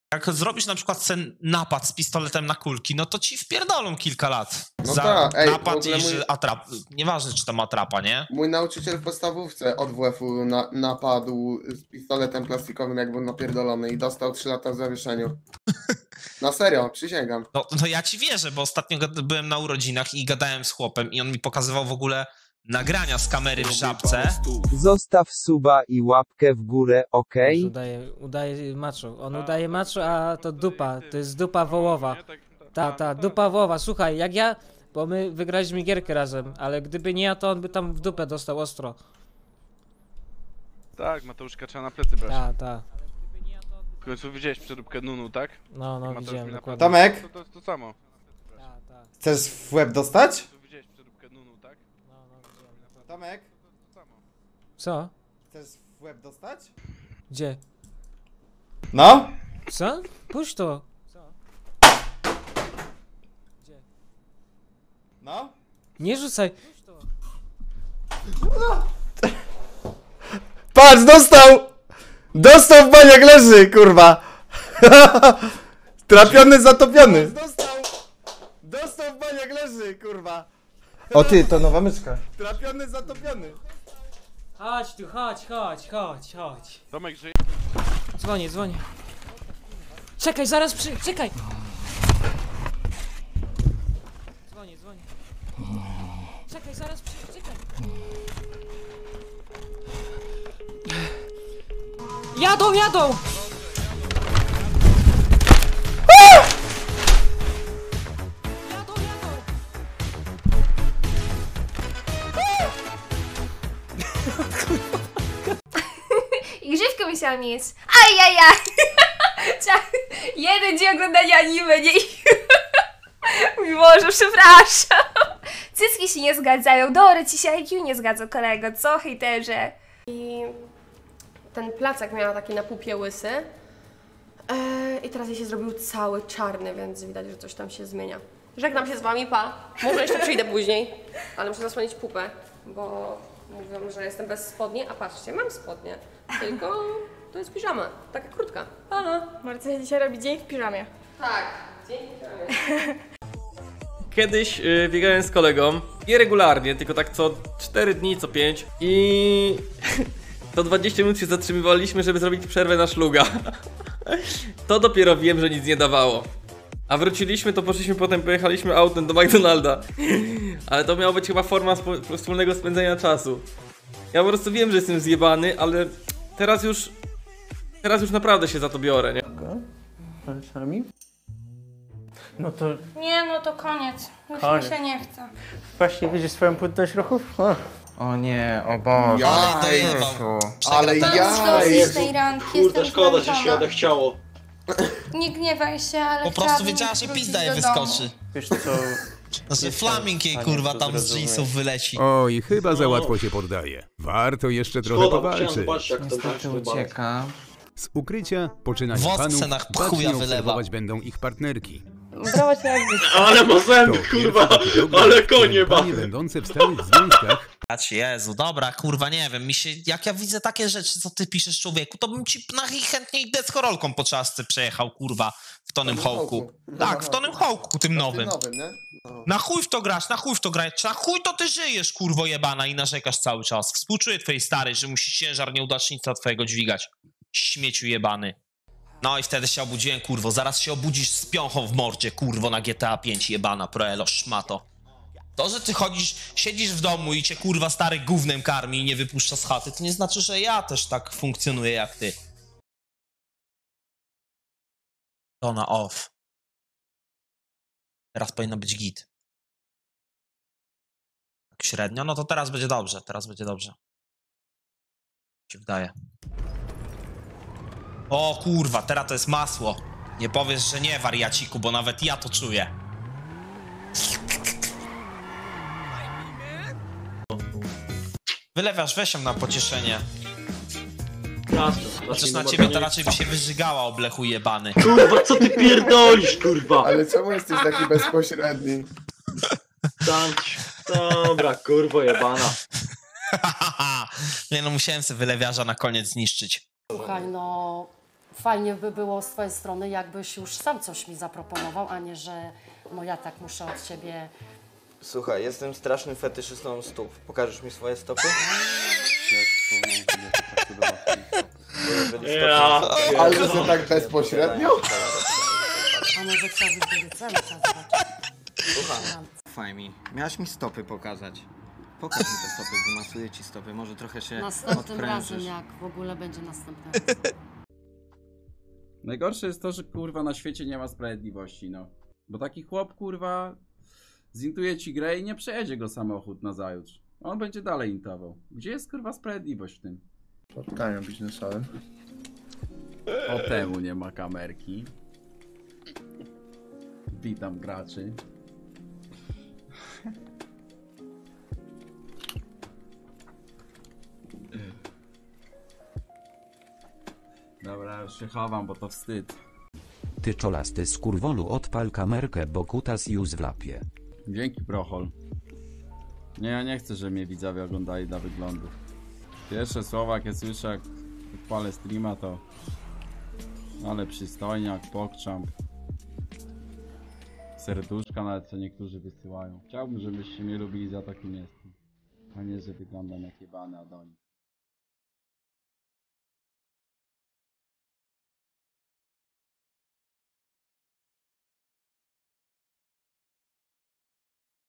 NIEUCZESANA WYJAŚNIA CHEJTERA, PAGO RZUCIŁ MYSZKĄ W TOMKA(Sagan świruje) - TWITCH SHOTY
Damy radę dobić 13 łapek w górę?
👉Zostaw like i subika z dzwoneczkiem🔔 aby być na bieżąco👈